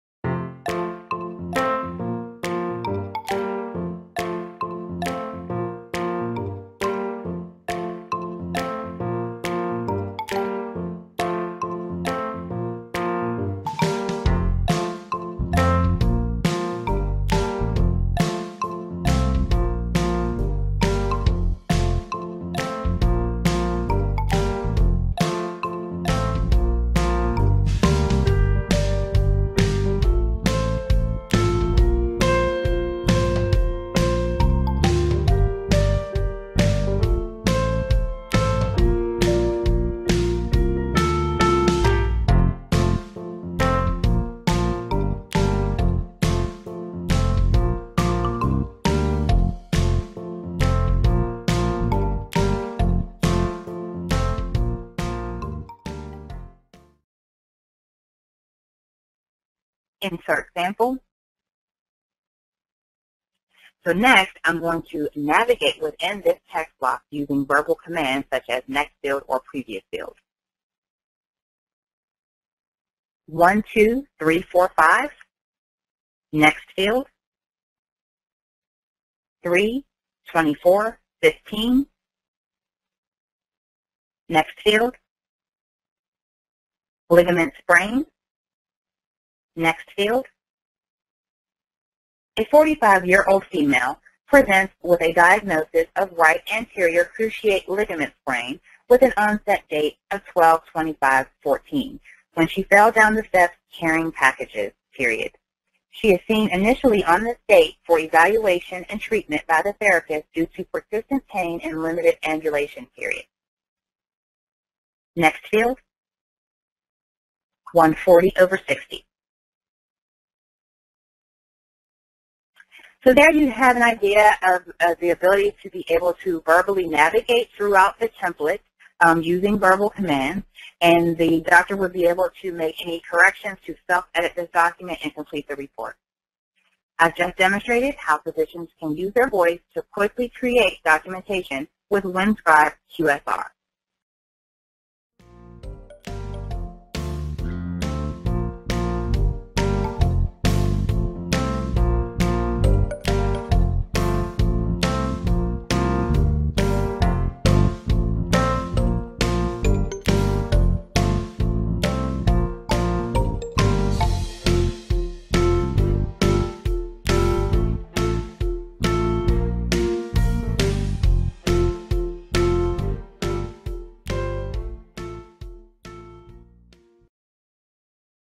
Winscribe QSR: Create Clinical Documents with Your Voice
This video provides an introduction to Winscribe Quick Speech Recognition (QSR) for healthcare professionals, as well as a demonstration showing Winscribe QSR's functionality to create medical documentation within Microsoft Word.